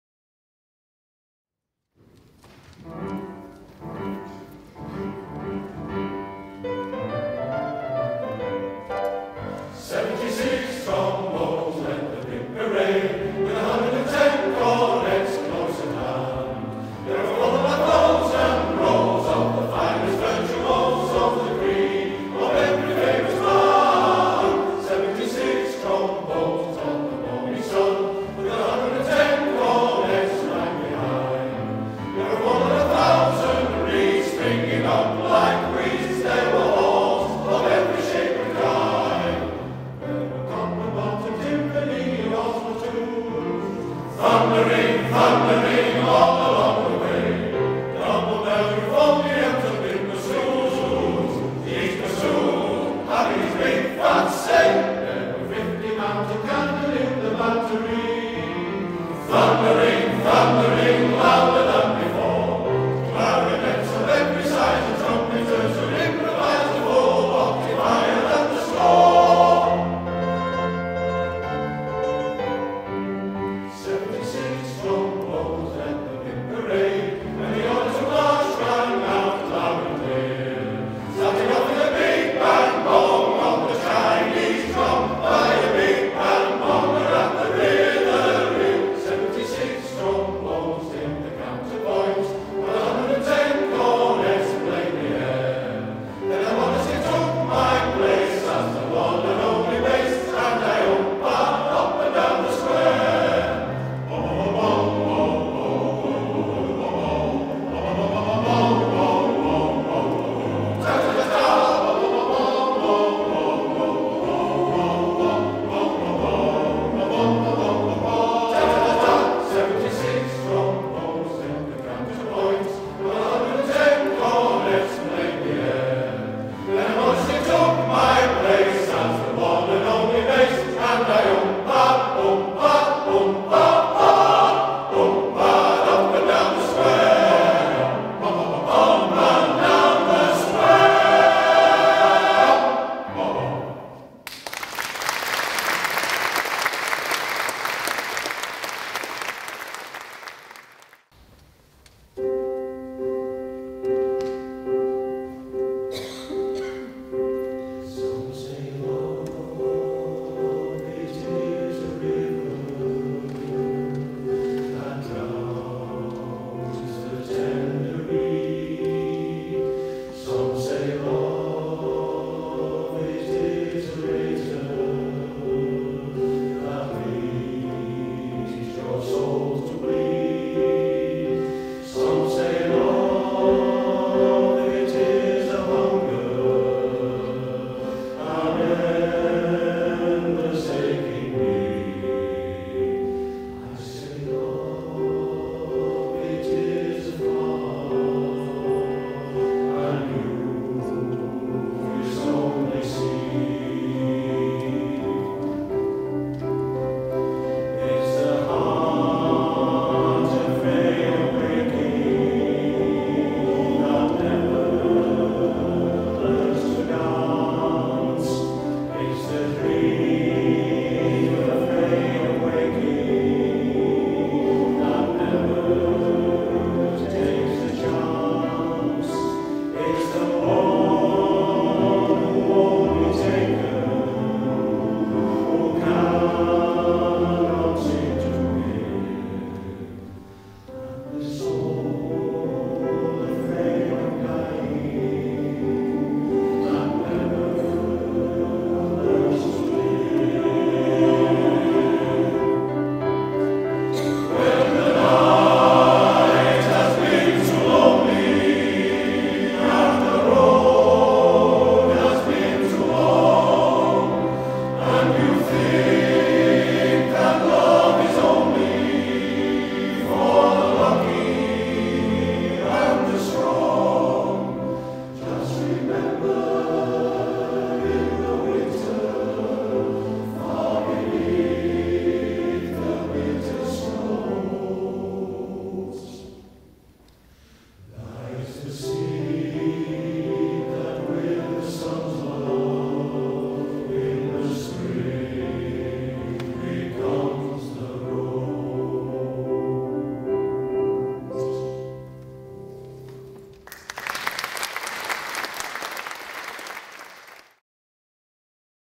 Hopes and Memories
Here is a short preview of a concert we were privileged to be invited to record in Wakefield Cathedral just prior to the Internal refurbishment.